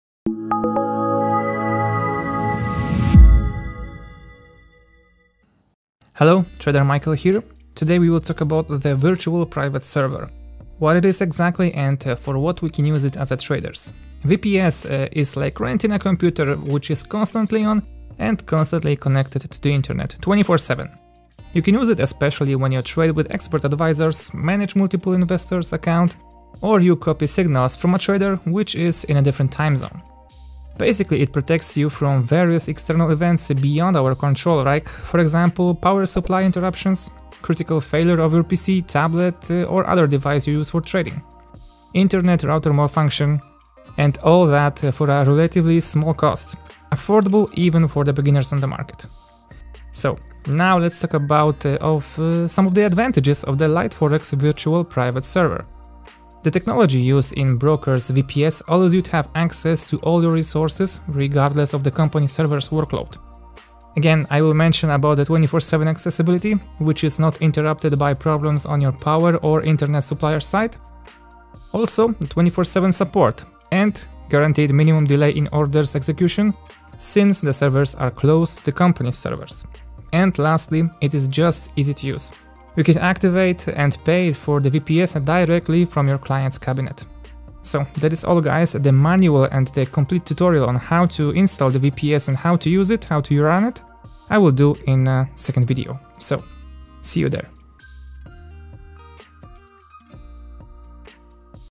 What is VPS service in forex and how to get it? | Liteforex tutorial
Hello! Trader Michael here! In this video, I'll explain what is VPS and why you should consider buying a VPS if you're serious about your trading. All that is a very short, simple, and enjoyable format. Hope you'll like this one.
Risk Warning: Trading on financial markets carries risks